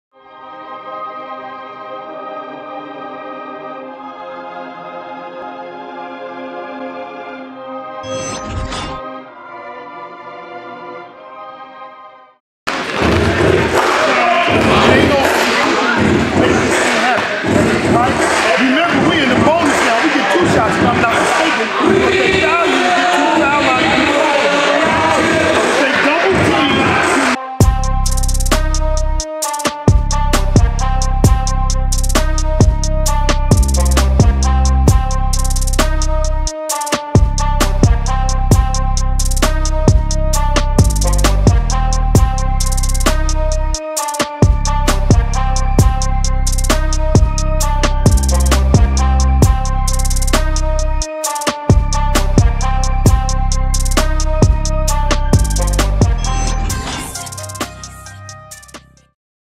BAA Tip-Off Classic
The BAA Tip-OFF classic features several school from the Philly area. BAA was established 5 years ago with an emphasis on athletic enrichment and providing a safe haven for young student-athletes.